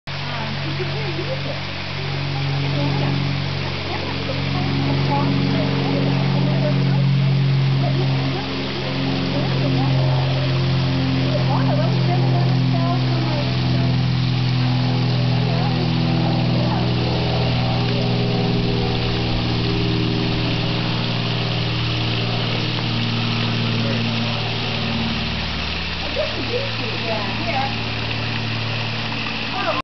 Swimming Hole At White Lotus Foundation
At the White Lotus Foundation in the mountains above Santa Barbara, there runs Skumuwash Creek. On this creek there are two swimming holes. This short video was shot on Labor Day 2008.